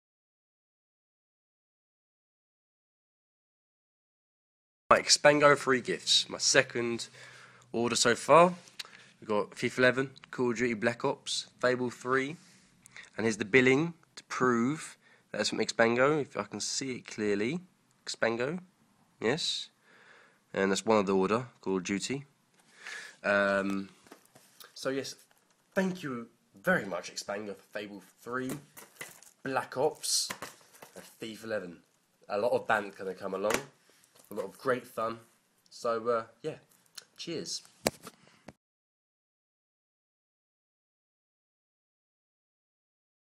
Max Hooper and his Free Xbox 360 Games from Xpango!
Here's Max Hooper and his Free Xbox 360 Games from Xpango, including Call of Duty Black Ops, Fable 3 and Fifa 11!